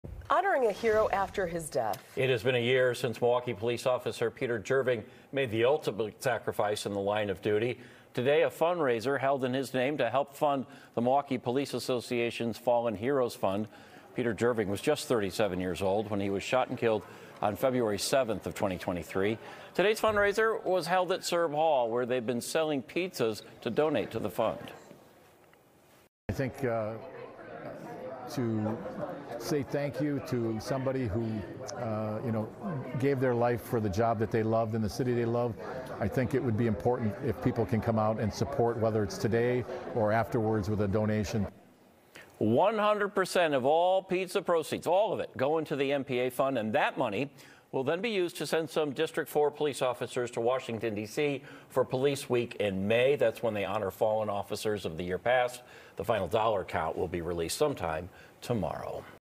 Police fundraiser honors fallen officer | FOX6 News Milwaukee
There was a fundraiser to honor fallen officer Peter Jerving and to help Milwaukee officers go to National Police Week in Washington, D.C. on Sunday, Feb. 18.